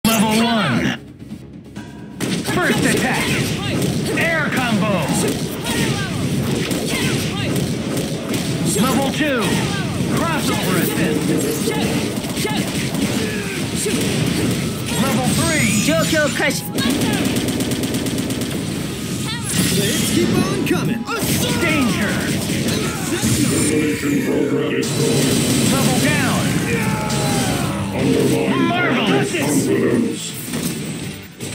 Finally Hitting max damage with Cammy
Also, make sure to comment so I may put more Team request videos out there!

I don't have any sponsors so please use this link for a discount on DoorDash(We'd both get a discount)